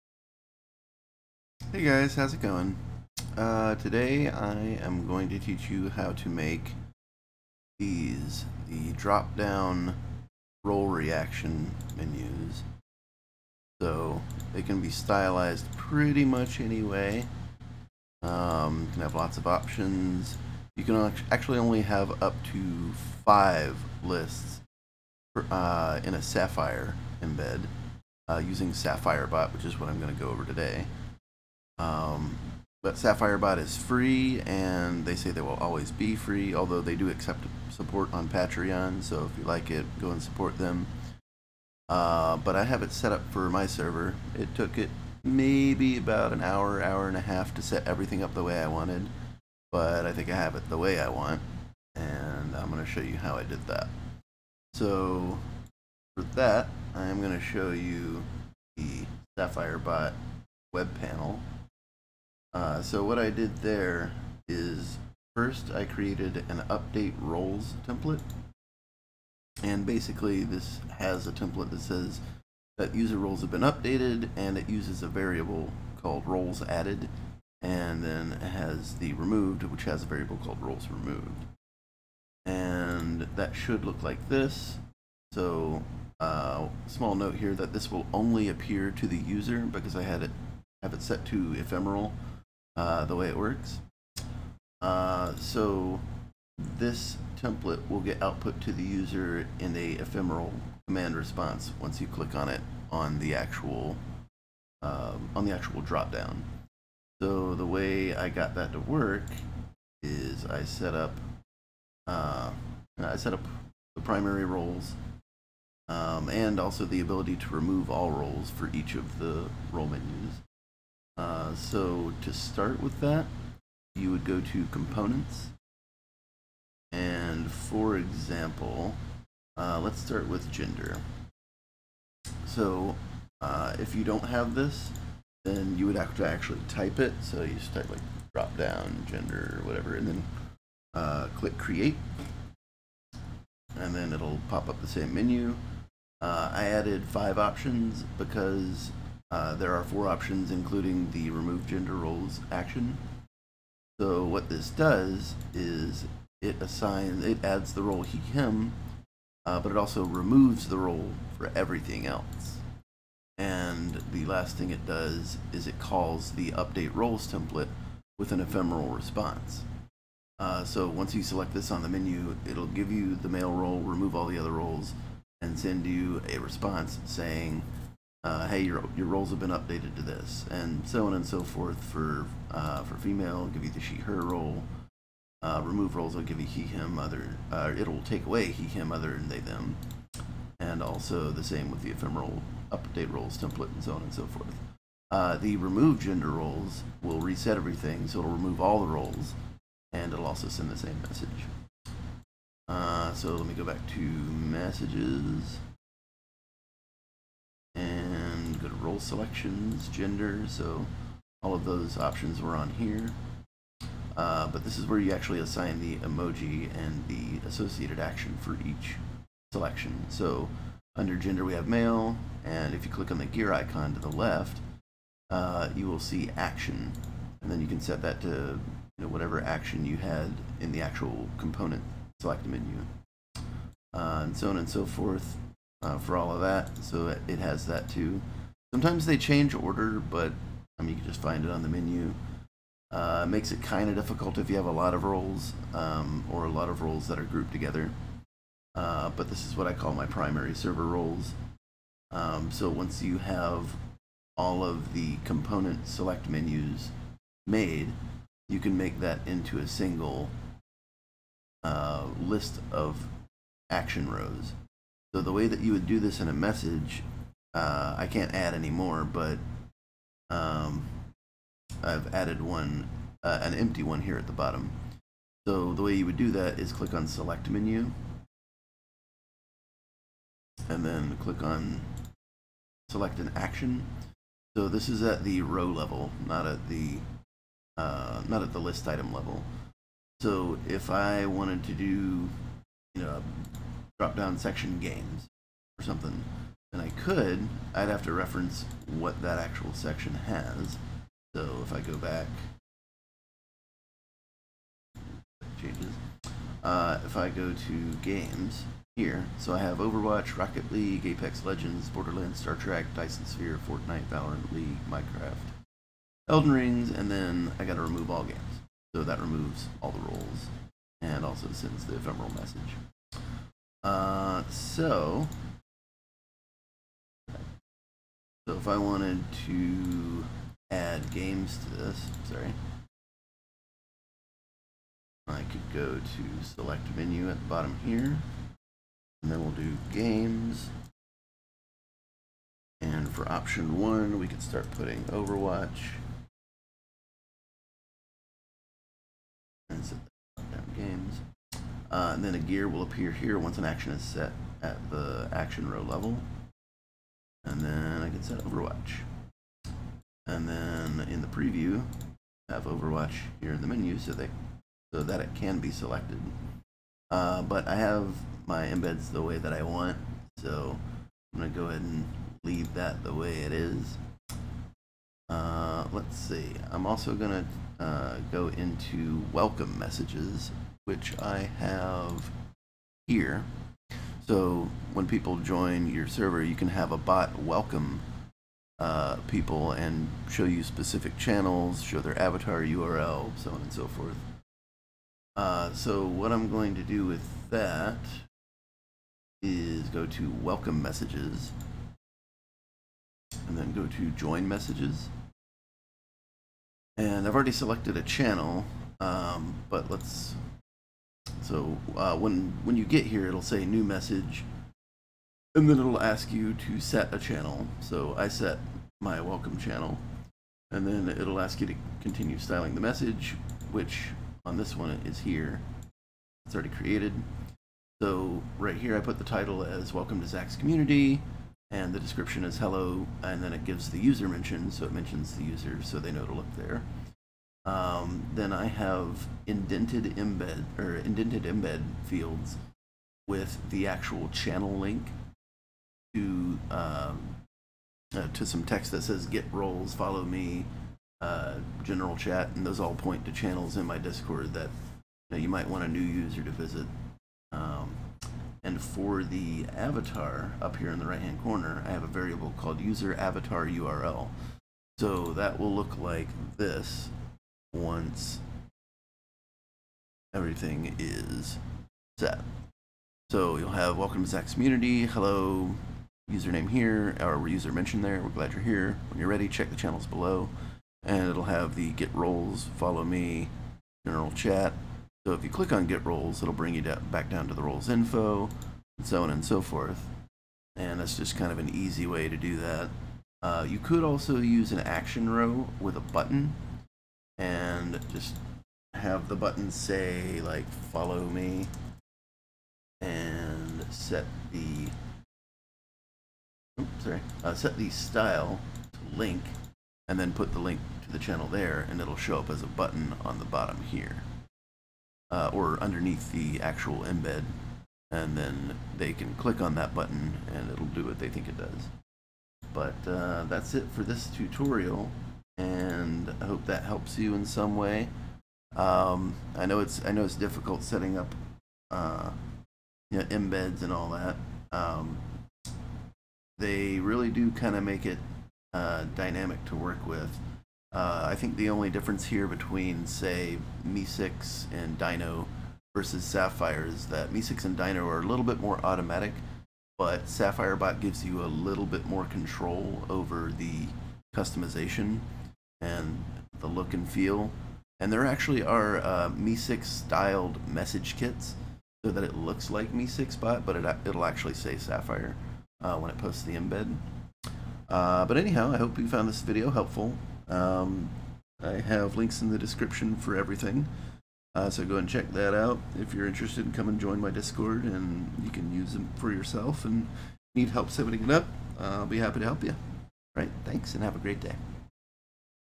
How to set up Discord welcome messages and role menus with Sapphire
This video shows you how I set up Sapphirebot to have an embed with role select menus.